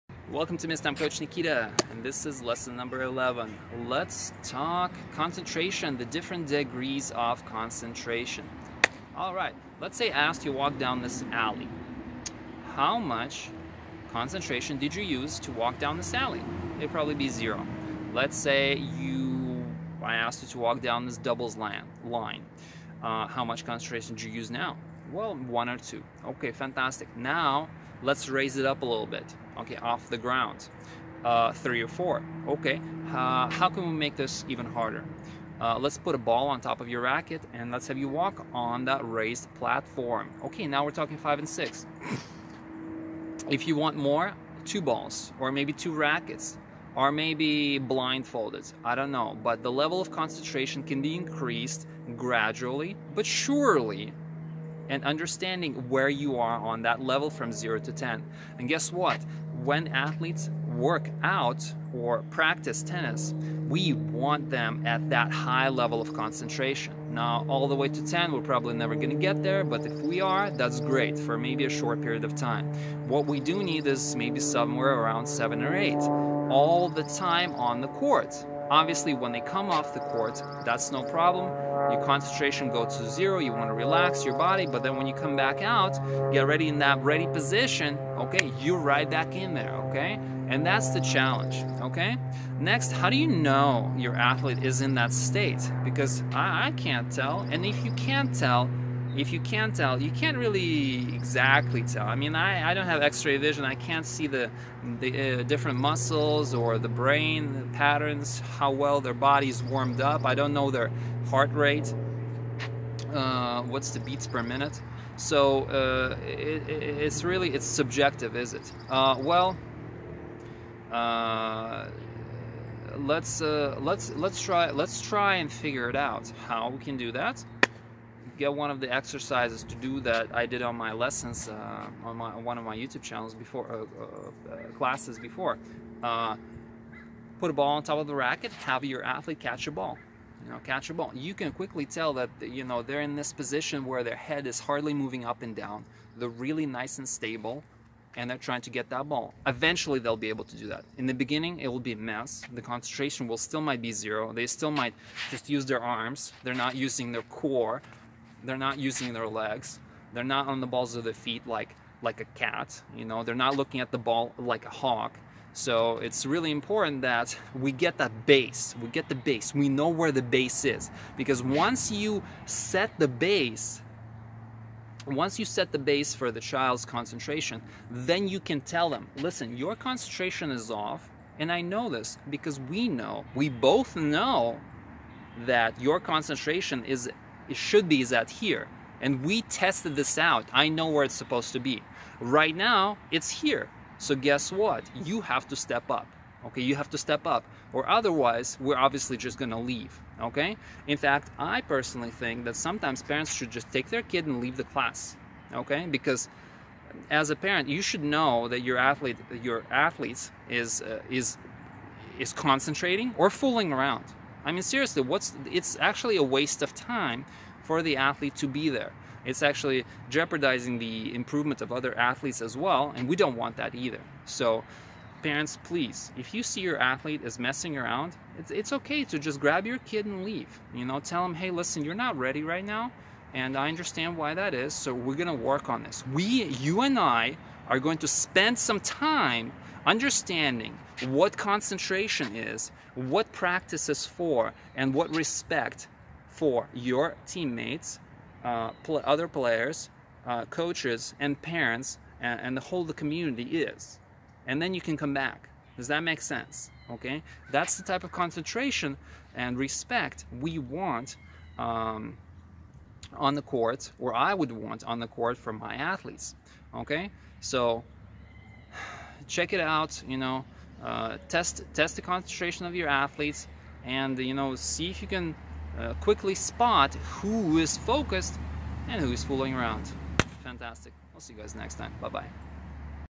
MIST - Lesson 11 - Degree of concentration - Tennis - Green Ball
Welcome to Mission Impossible School of Tennis!

In today's lesson 11: Degree of concentration